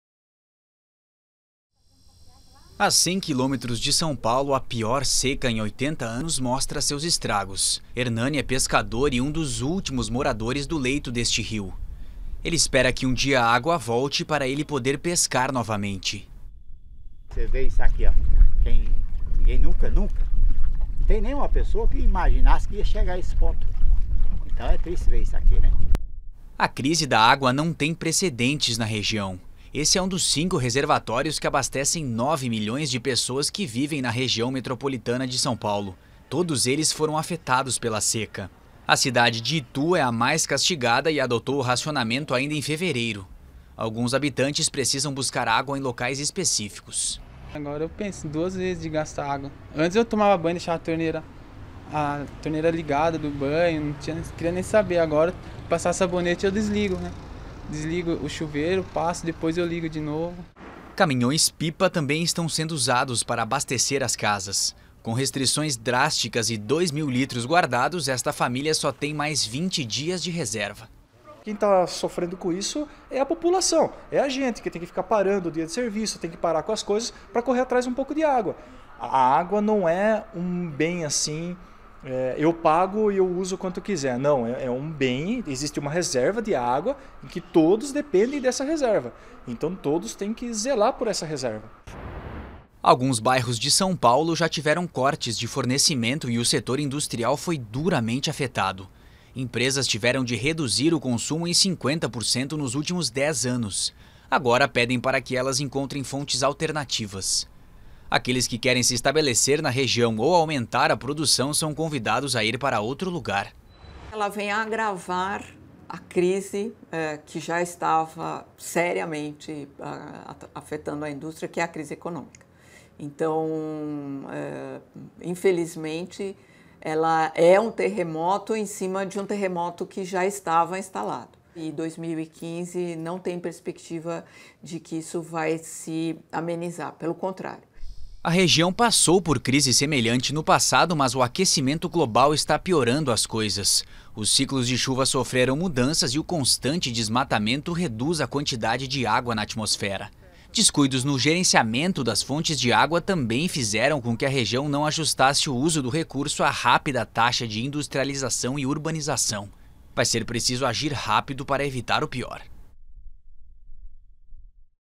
As várias faces da crise da água em São Paulo
A seca em São Paulo, centro econômico do Brasil, traz problemas tanto para as famílias como para as indústrias. Somada às causas naturais, estão problemas de gestão deste recurso essencial.